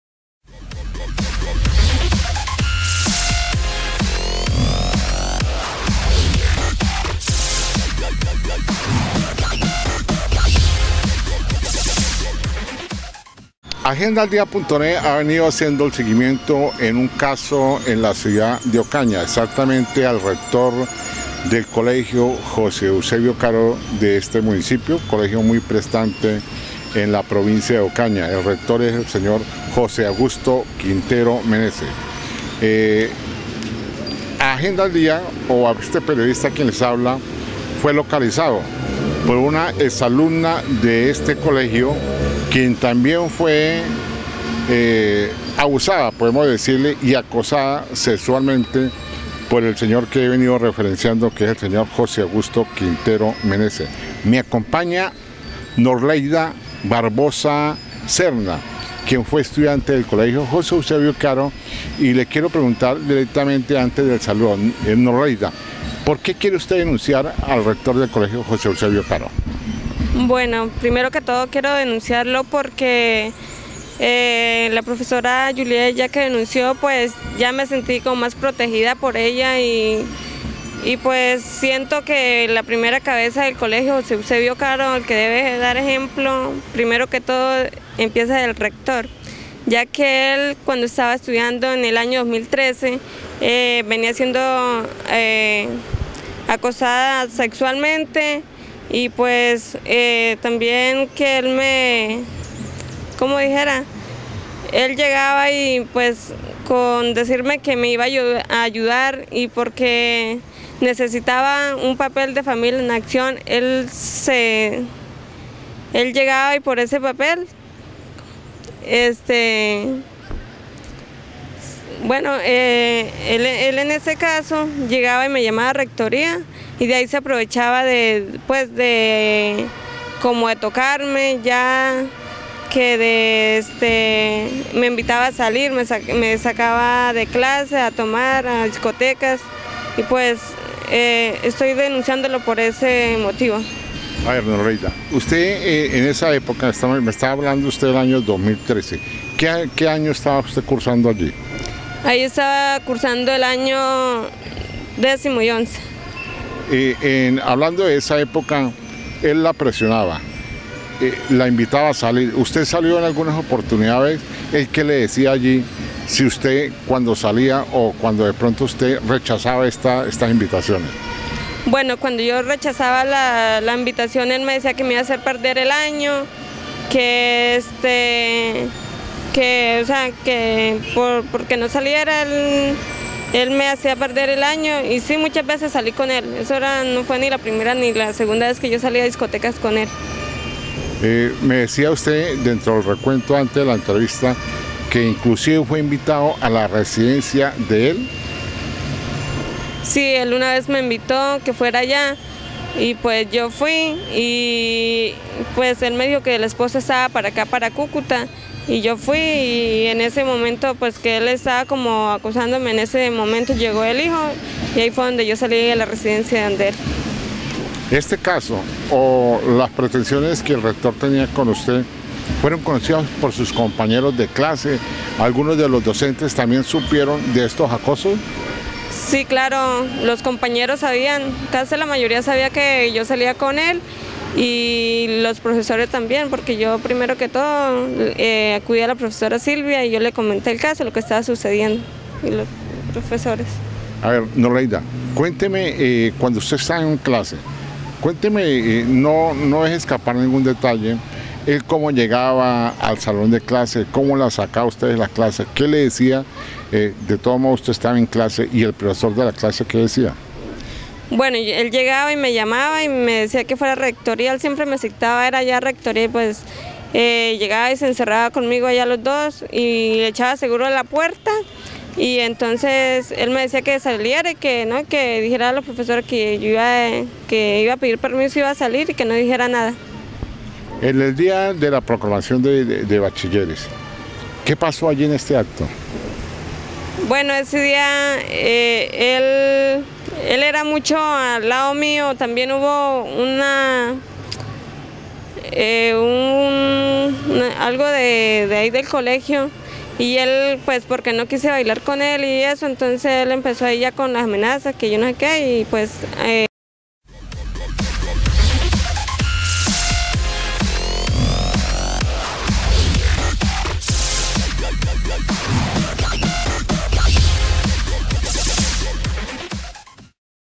NORLEIDA BARBOSA EX ALUMNA DEL COLEGIO JOSE EUSEBIO CARO DE OCAÑA
JOSE AUGUSTO QUINTERO MENESES RECTOR DEL COLEGIO JOSE EUSEBIO CARO DE OCAÑA ES DENUNCIADO POR LA  EX ALUMNA  NORLEIDA BARBOSA SERNA, QUIEN DICE QUE EL RECTOR LA ACOSABA SEXUALMENTE.